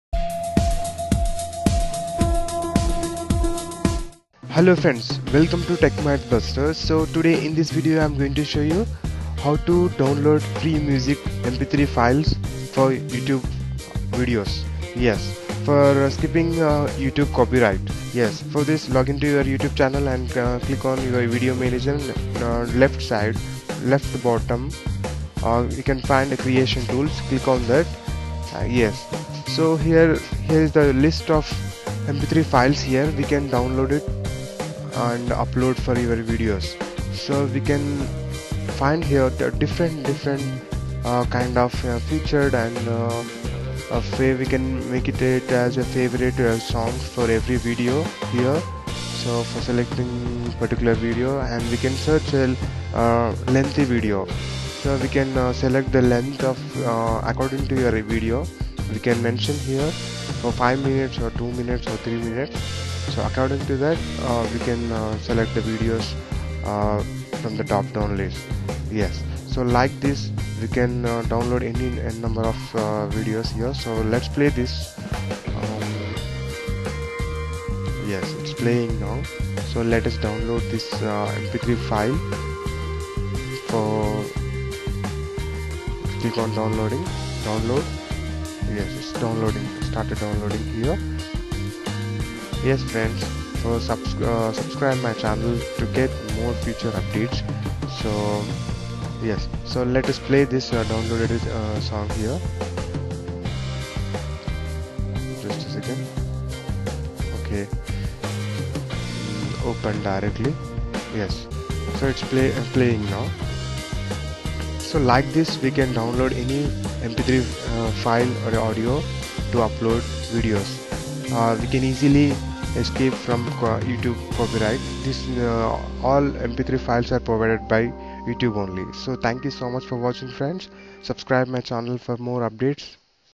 Free Download Background Music for YouTube Videos-TMB
In this videos We All Learn How to Download Free Music/Audio/Mp3/Background for YouTube Videos to Skip or Escape From You Tube Copy right. so we can download Official Music From you tube.